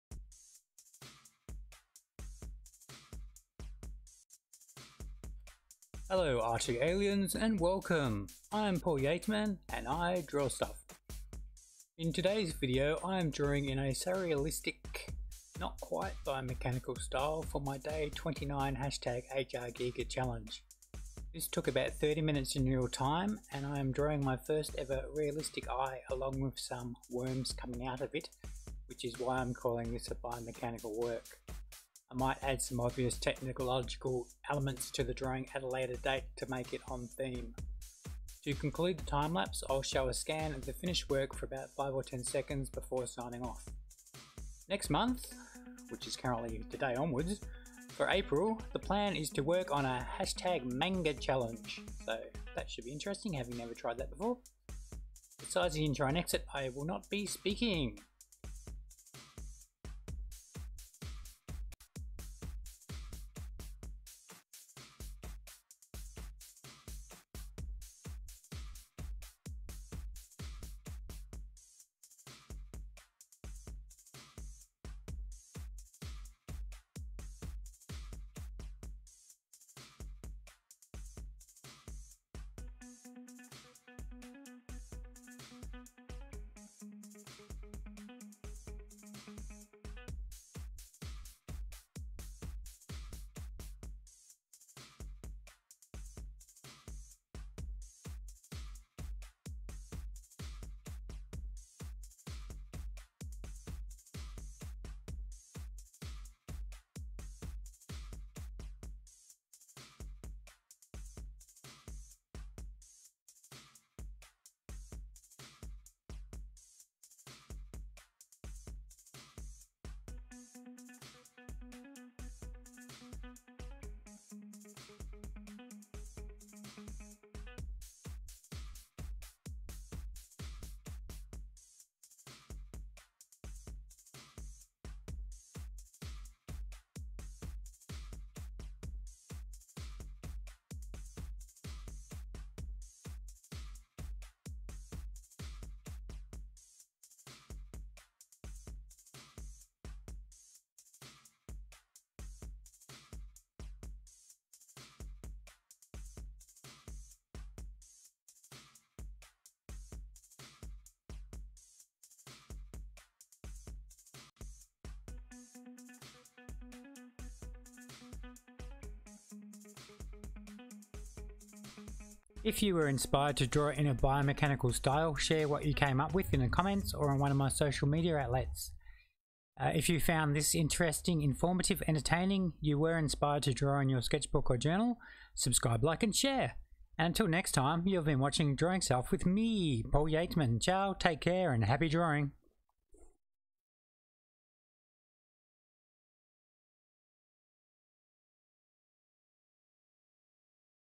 Realistic Eye | Draw In a Biomechanical Style
In today’s video I am drawing in a surrealistic not quite biomechanical style for my day 29 HRGigerChallenge. This took about 30 minutes in real time and I am drawing my first ever realistic eye along with some works coming out of it which is why I’m calling it a biomechanical work. I might add some obvious technological elements to the drawing at a later date to make it on theme. To conclude the time-lapse I’ll show a scan of the finished work for 5-10 seconds before signing off.

For April the plan is to work on a MangaChallenge

Besides the intro and exit, I will not be speaking.

CHAPTERS
0:00 Intro Screen
0:20 Sketching
1:55 Adding Line Art and Colour
2:50 End Screen

Get social with me...